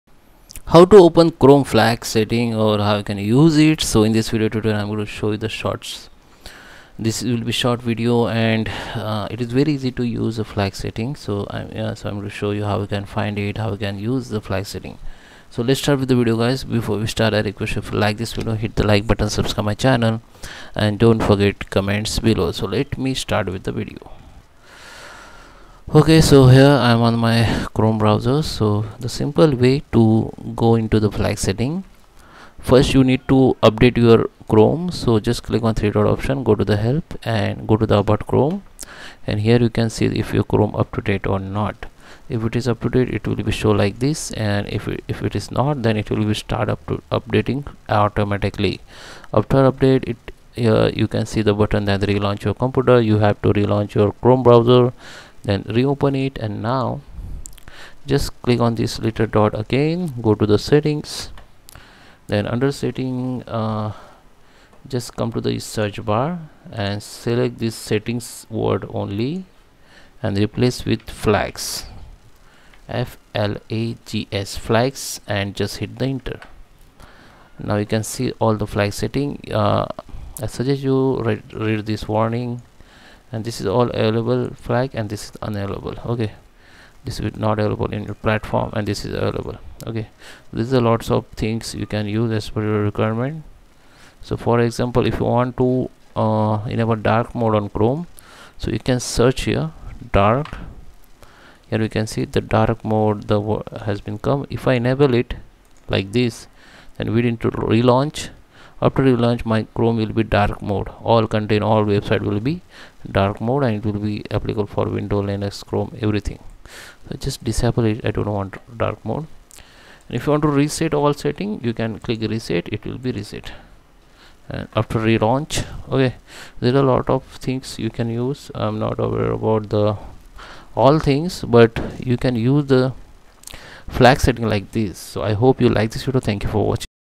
How to Open Chrome Flags | How to Use Chrome Flags
how to open chrome flags! just just follow this tutorial and learn how to open google chrome flags.